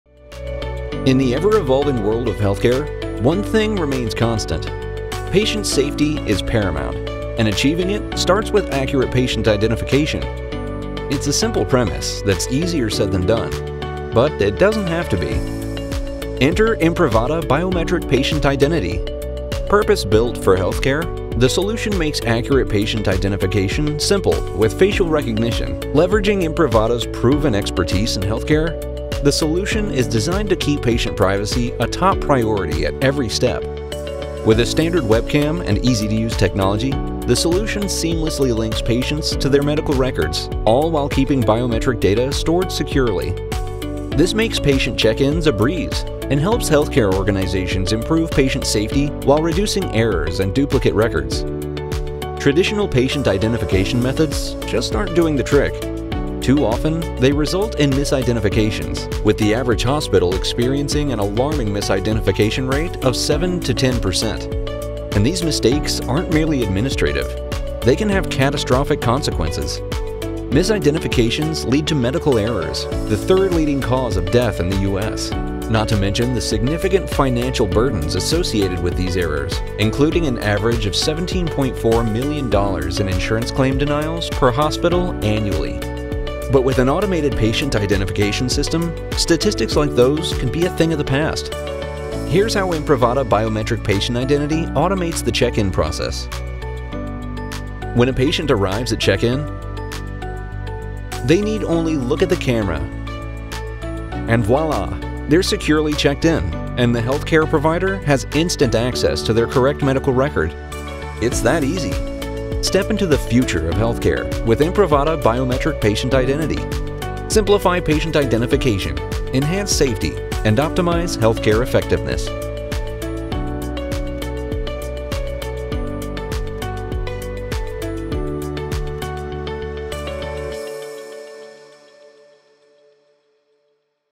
Imprivata Biometric Patient Identity: Simplifying Healthcare Check-ins and Enhancing Safety
In the ever-evolving world of healthcare, patient safety remains paramount. Accurate patient identification is crucial, and Imprivata offers a purpose-built solution designed specifically for healthcare settings. Leveraging facial recognition technology, this solution simplifies patient identification, ensuring privacy and security at every step. With a standard webcam and user-friendly interface, patients can seamlessly link to their medical records, streamlining check-ins and reducing errors. Traditional identification methods often lead to misidentifications, which can have catastrophic consequences. Imprivata's automated patient identification system eliminates these risks, improving patient safety and optimizing healthcare workflows. Step into the future of healthcare with Imprivata Biometric Patient Identity and experience simplified identification, enhanced safety, and optimized healthcare outcomes.

5 Reasons to watch
1. Understand the importance of accurate patient identification in healthcare and how it contributes to patient safety.
2. Learn about the Imprivata Biometric Patient Identity solution and how it simplifies the patient check-in process.
3. Discover how facial recognition technology can be leveraged to link patients to their medical records securely.
4. Gain insights into the potential benefits of implementing an automated patient identification system, including reducing errors and duplicate records.
5. Explore how the Imprivata Biometric Patient Identity solution can enhance safety, optimize healthcare processes, and improve patient identification in healthcare settings.